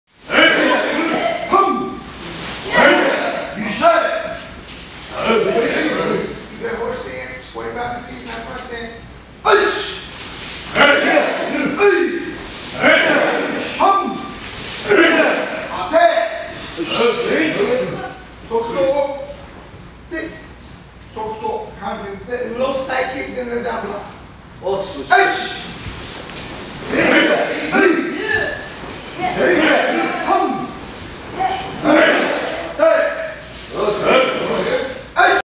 Training With Kancho Yoshiji Soeno At Shidokan Atlanta
Shidokan Karate founder, Kancho Soeno teaching a class at Shidokan Atlanta/Trammell Fitness and Martial Arts.